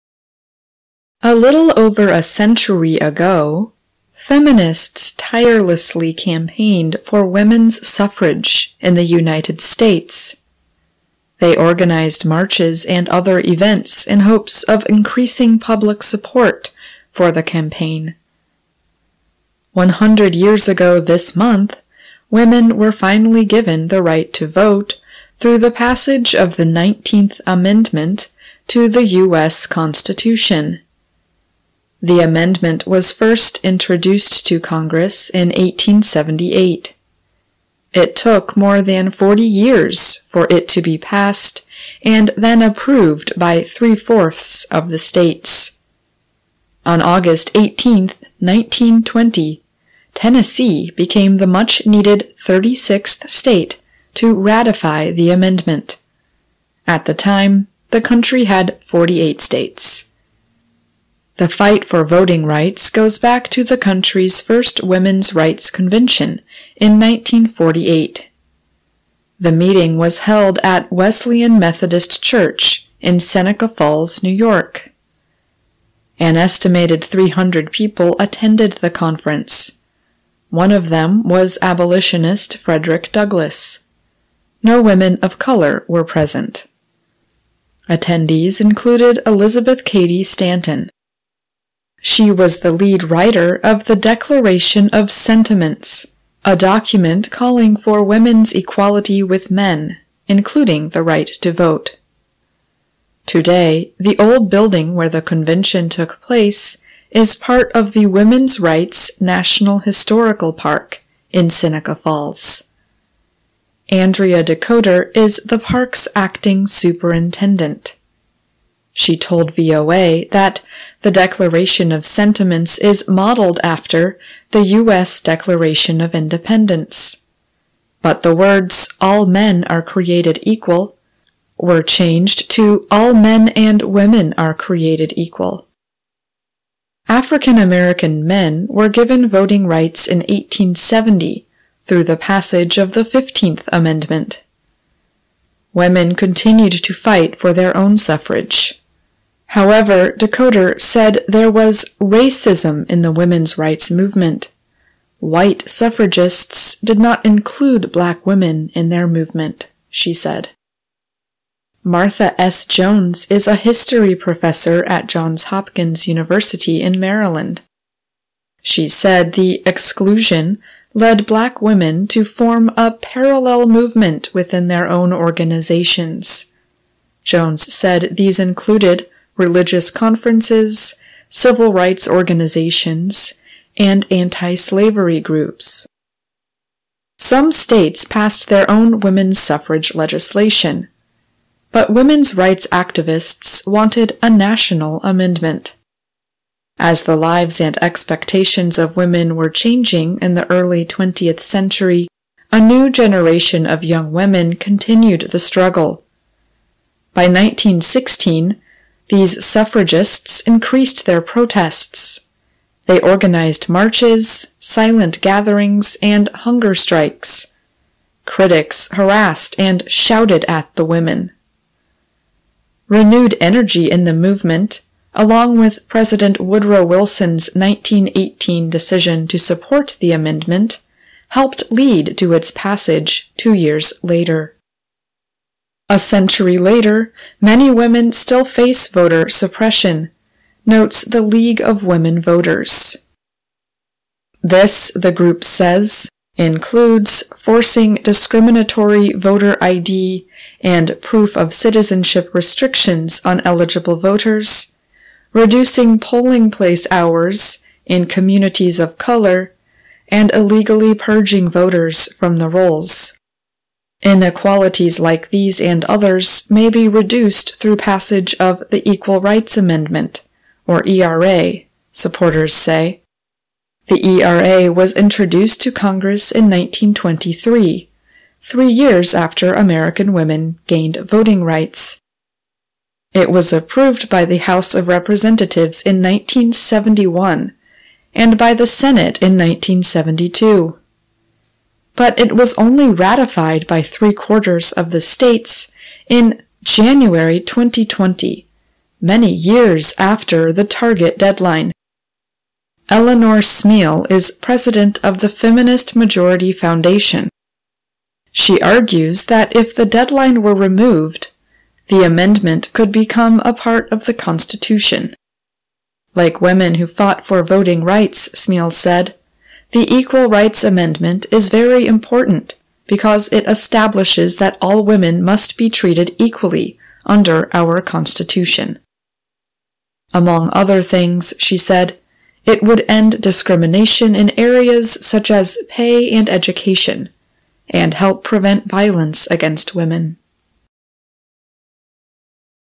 英文聽力大補丸 (288)：美國婦女擁有投票權已百年。US Marks 100 Years Since Women Gained Right to Vote
知識常識類：
英文教學類：
綜合性：